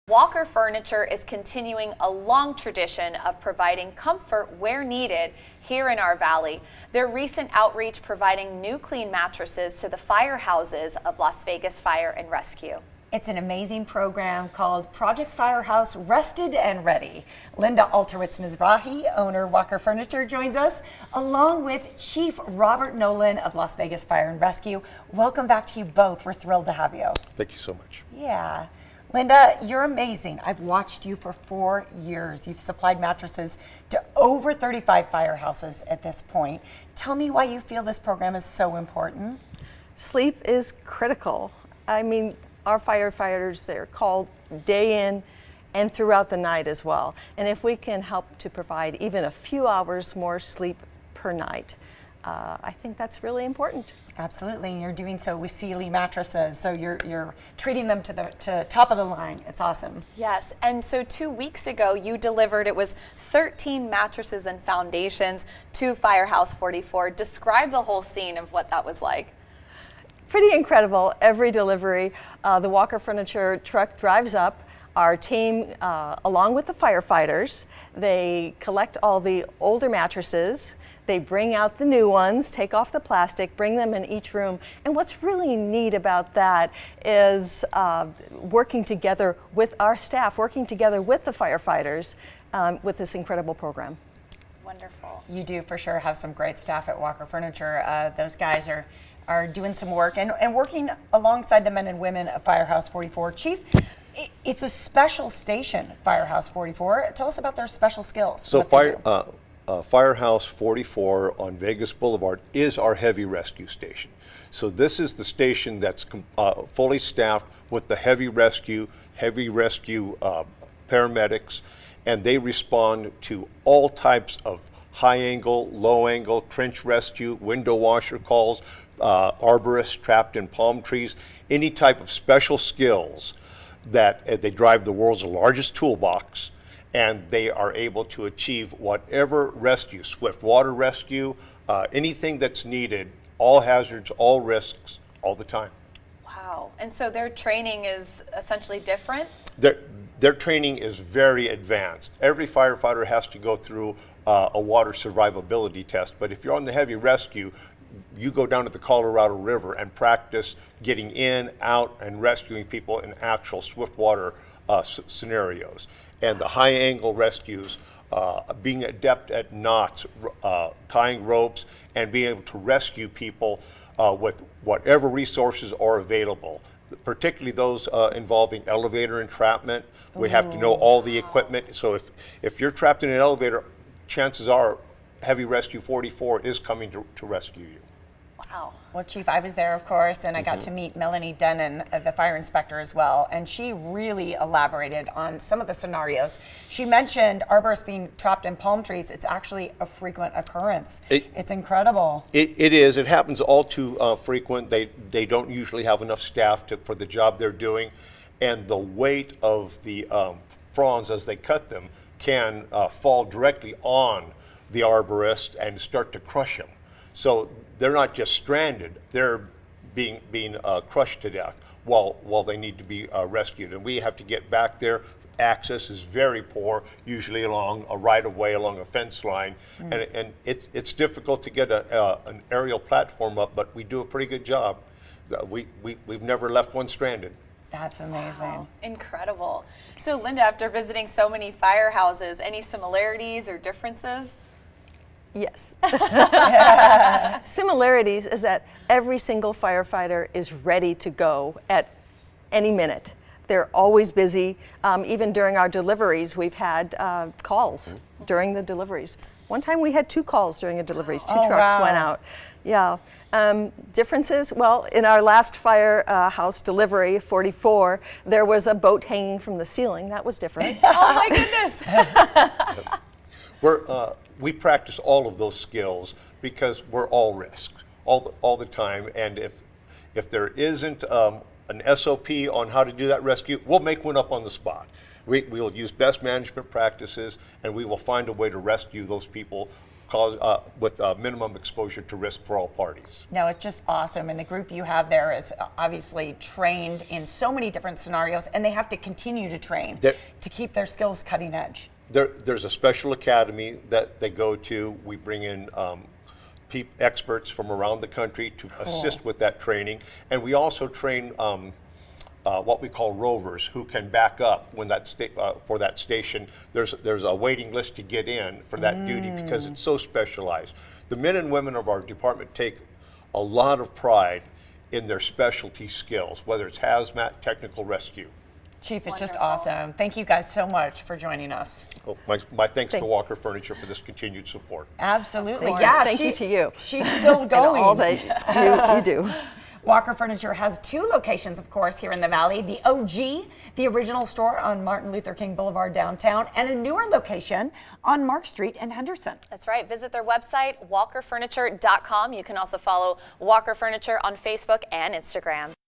Program Provides New Mattresses, Foundations To Fire Stations
Did you know first responders who use beds all through the day and night for intermittent sleep can go years before receiving new mattresses?